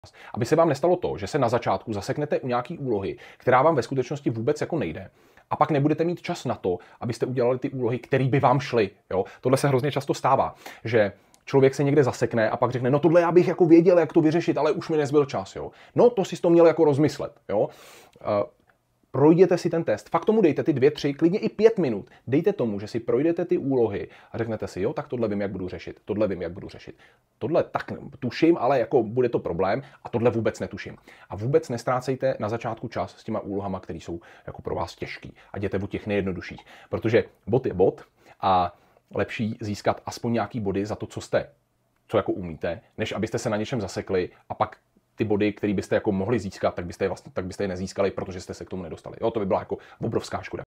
⁉️ OVLÁDNÍ ČAS u přijímačkových testů
Pracovat efektivně s časem je pro úspěch u přijímačkového testu klíčové. Jak na to Vám ve videu prozradí Marek Valášek.